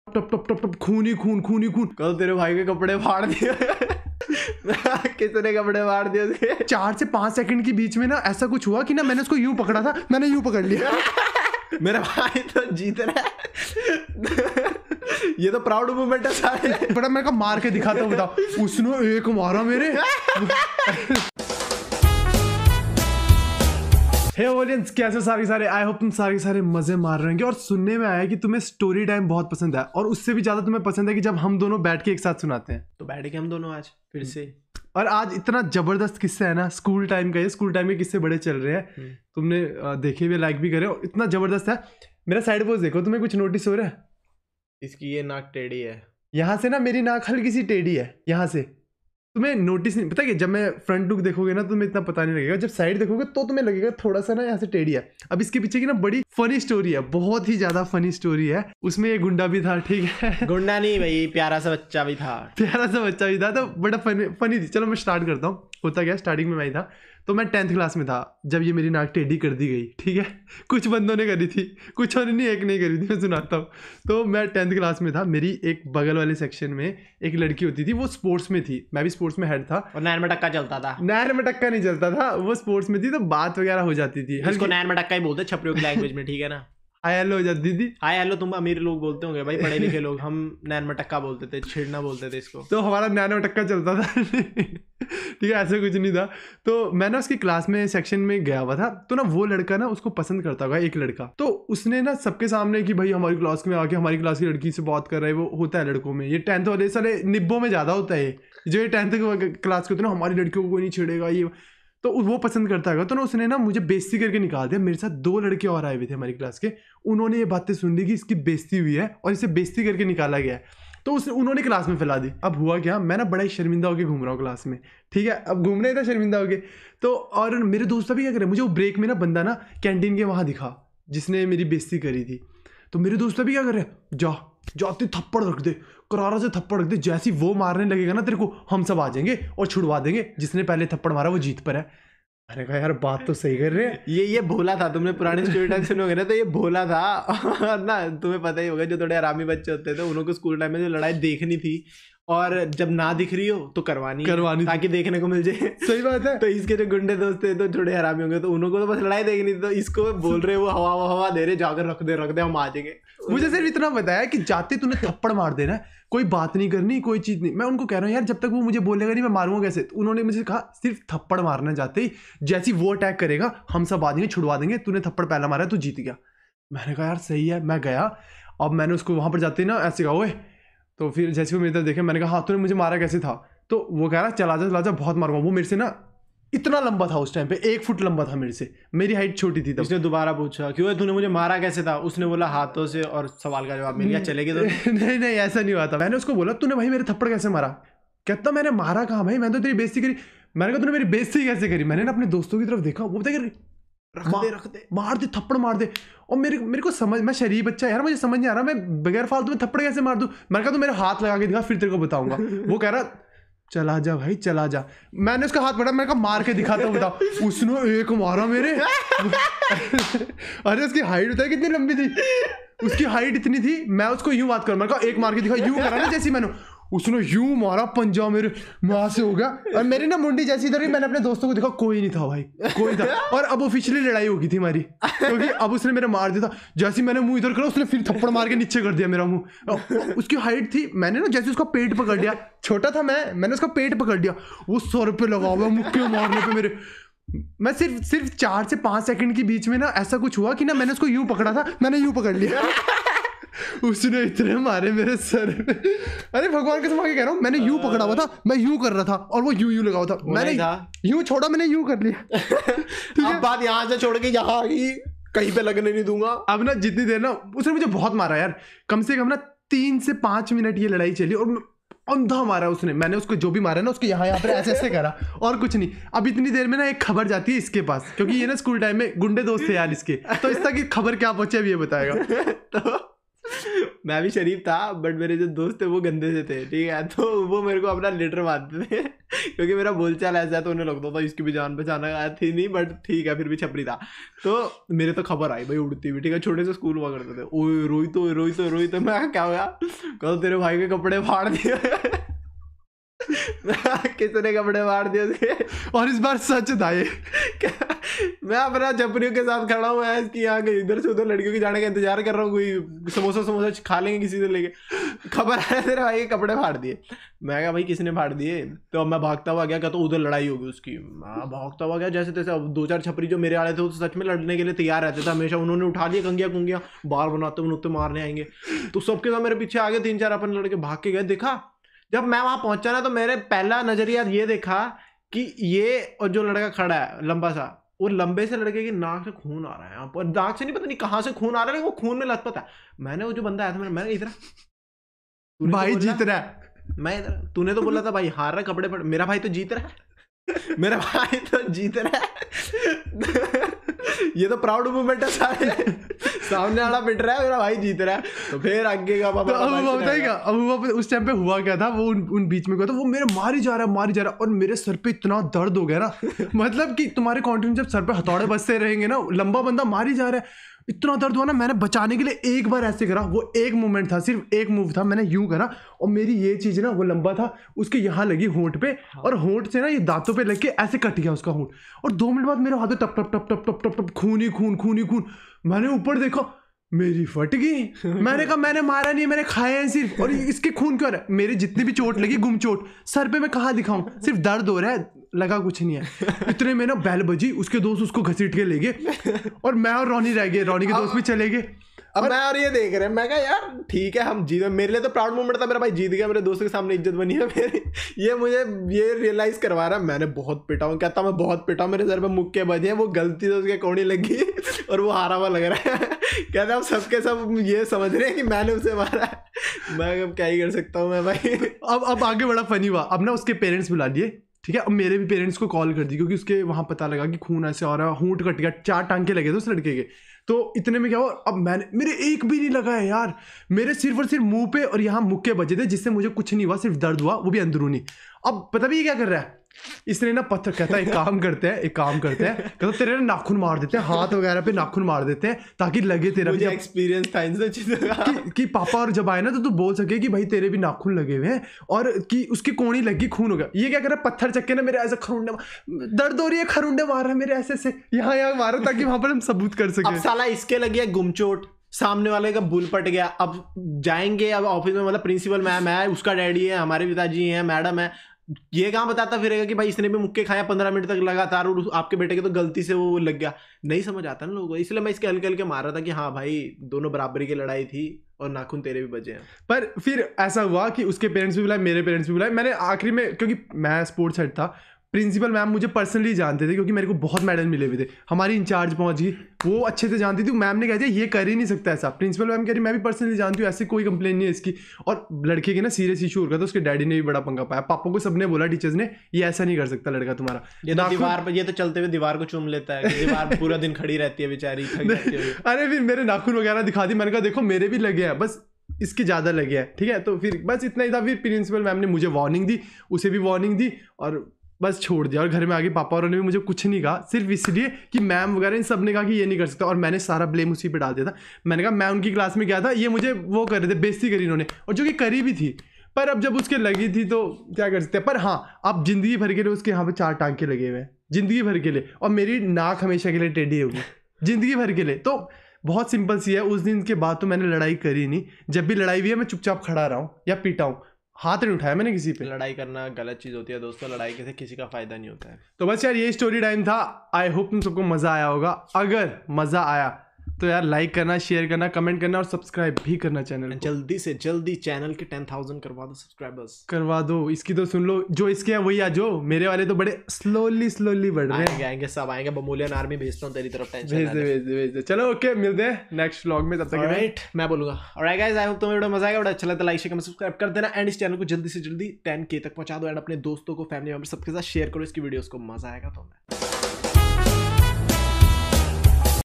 Ladko ne Naak Ki hadi Tod di😞 FT. @RohitBamola *LIFE TIME REGRET*🥺
Naak Ki hadi Tut Gai FT. @Rohitbamola *LIFE TIME REGRET*
                                    Ankitbamola
hello doston
     HASTE RHO MUSKURATE RHO